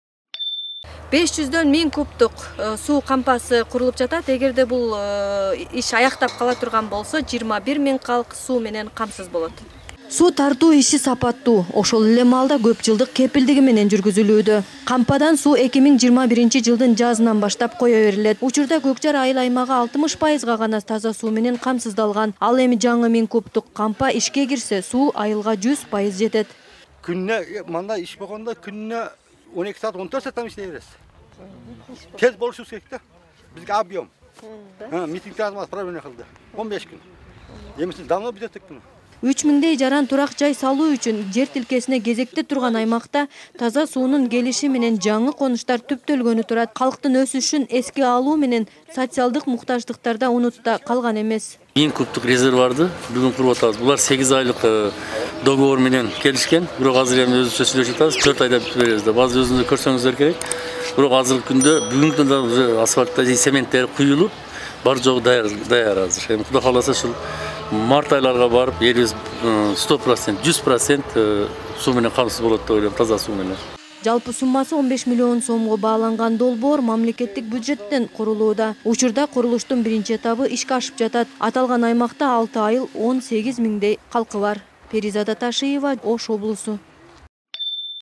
Ош: Көк-Жарга суу кампасы курулууда
Оштун Ноокат районунун Көк-Жар айыл аймагын таза суу менен камсыз кылуу үчүн таза суу кампасы курулууда. Миң кубдук кампанын курулушу менен айылдагы 2,5 миң кожолуктун ичүүчү таза сууга болгон муктаждыгы жоюлат.